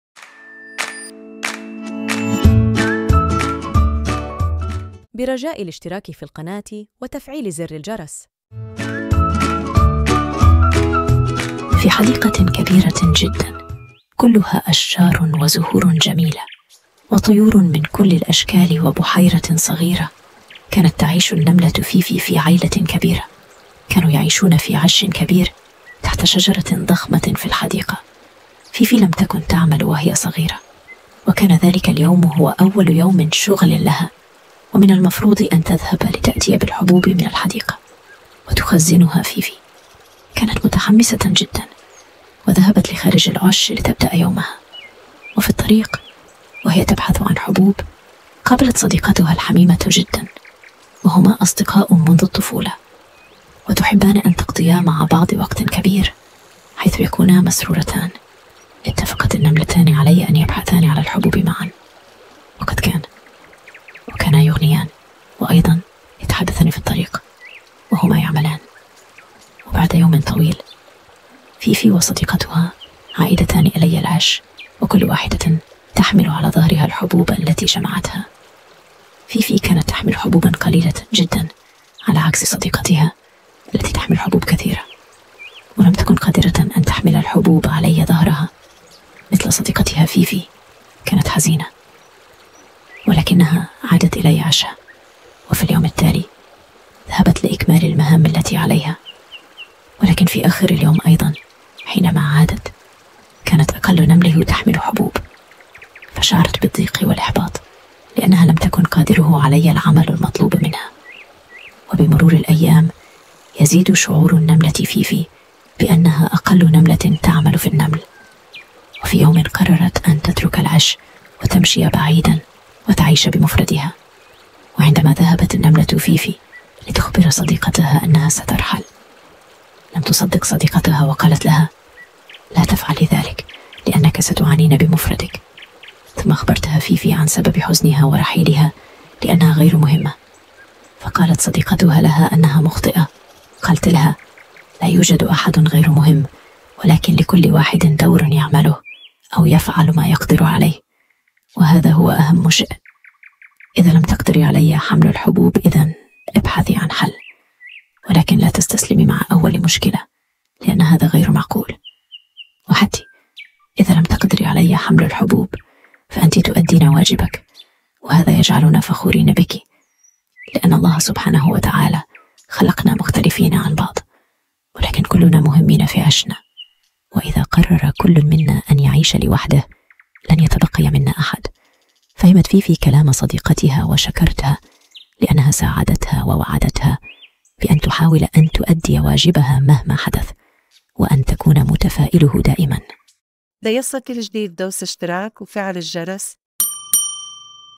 كيف ساعدت سوسو فيفي في حل مشكلتها مع النمل؟ قصة النملة فيفي .
قصة عن قيمة العمل و الصداقة و التفاؤل

فيفي هي نملة صغيرة ولطيفة تحب العمل والمساعدة في قرية النمل. لكنها تواجه مشكلة كبيرة: هي ضعيفة جداً ولا تستطيع حمل الأغذية الثقيلة مثل بقية النمل. تشعر فيفي بالحزن والإحباط وتفكر في ترك القرية والبحث عن مكان آخر. لكن صديقتها ترى معاناتها وتقرر مساعدتها. تأتي بفكرة رائعة و تنجح الفكرة وتصبح فيفي سعيدة ومفيدة لقريتها. =

قصة النملة فيفي  قناة حكاية للأطفال